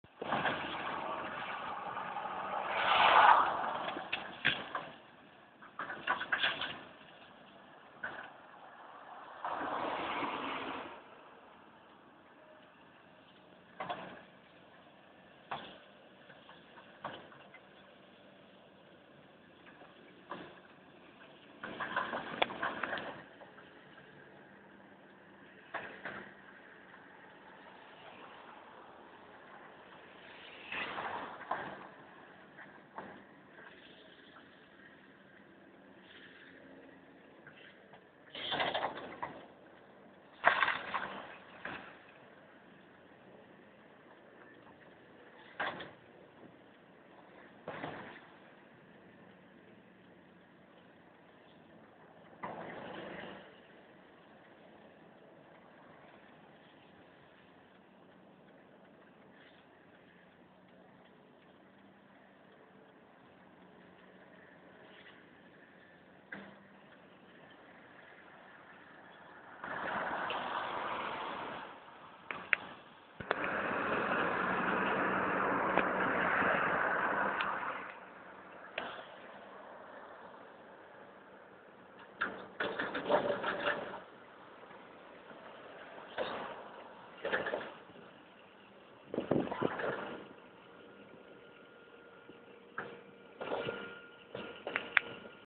Tearing Down Cheshire
Middle of town strip mall getting smashed to make room for yet another pharmacy.